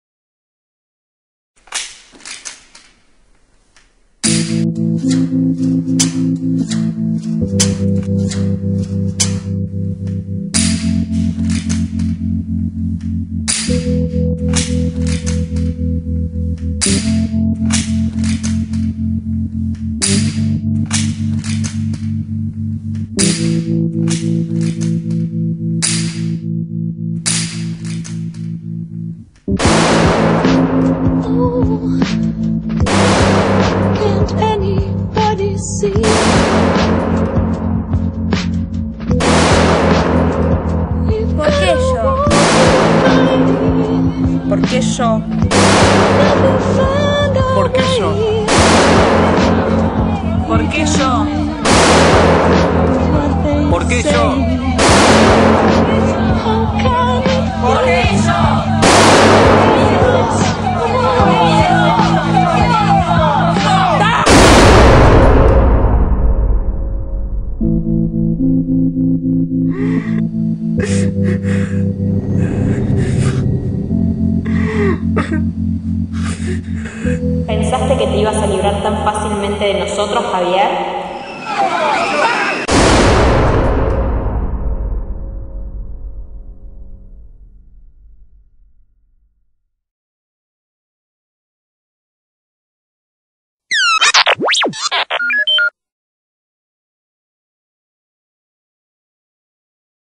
Bang Bang Estás Muerto [Trailer]
Funboy Prods presenta el trailer de la obra *¡Bang, Bang! Estás Muerto*, realizada por el Grupo INDECISOS,